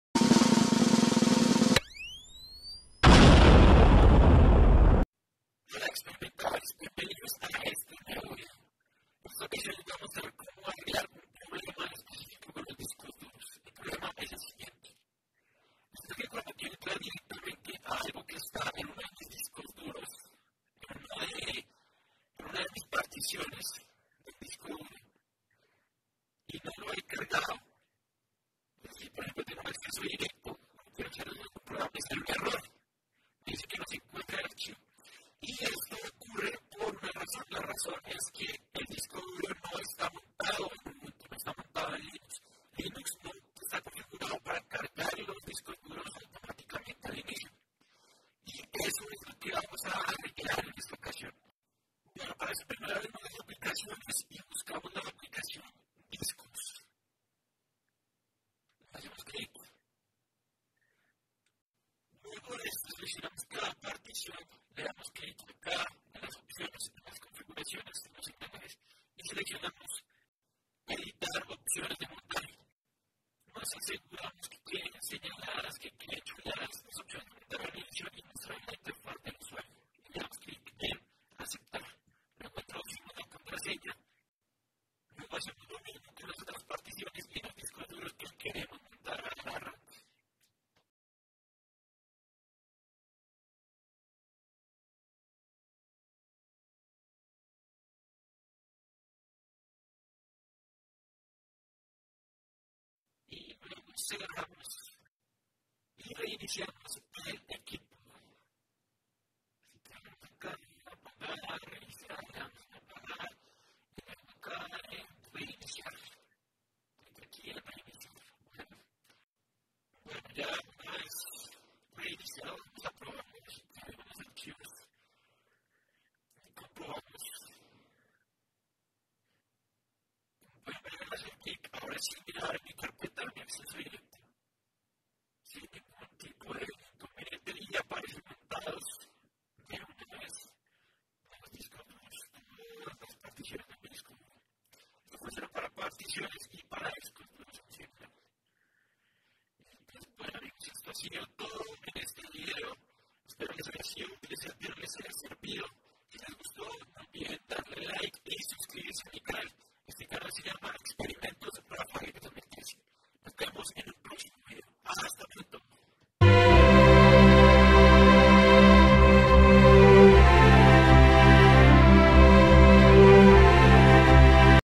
Como montar discos automaticamente en ubuntu 20.04
en este video cuento como solucionar el problema de que los discos duros o particiones no se  pueden acceder  por que no estan  montados desde el comienzo en  ubuntu.